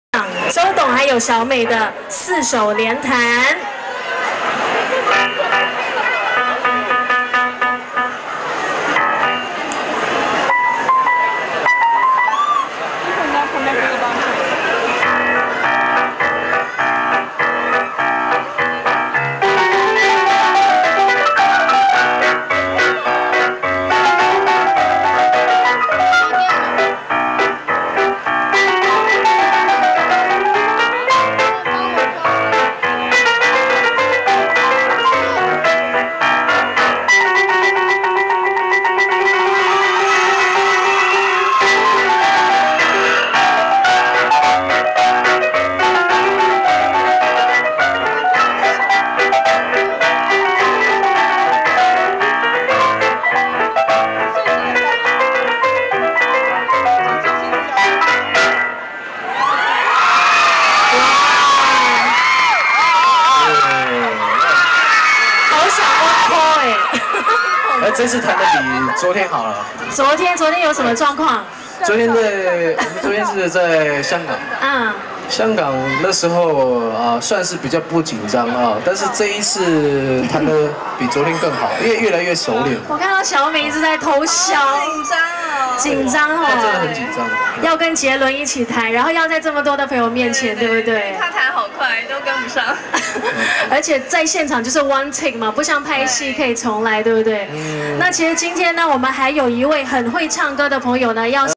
Jay and Kwai Lun-Mei Playing Piano @ Cathay
not really that clear..ps.. got pple blocking